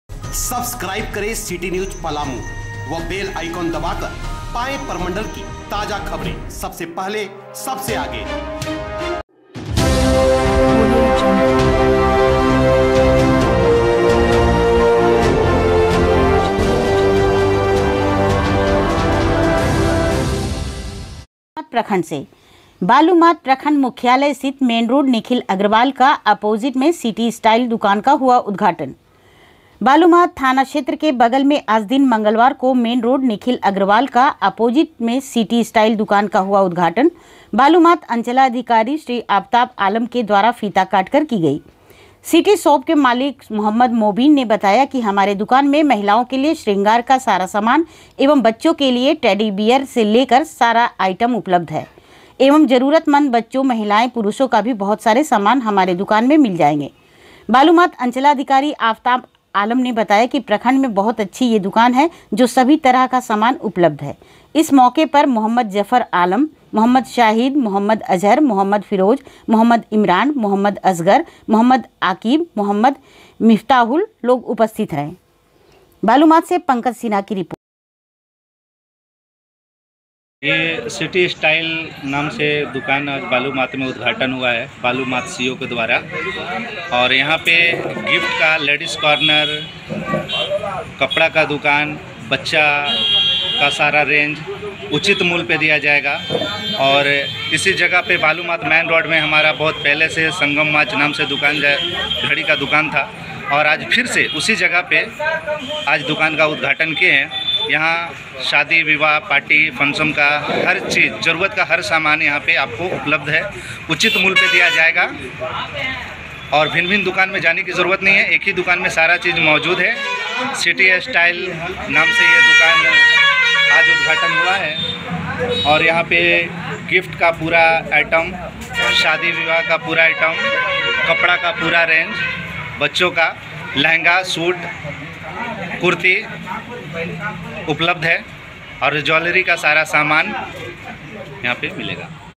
लातेहार--सिटी स्टाइल नामक प्रतिष्ठान का हुआ उद्घटान
बालूमाथ से पंकज सिन्हा की रिपोर्ट                                                                                       ''This channel and I do not claim any right over any of the graphics, images, songs used in this video.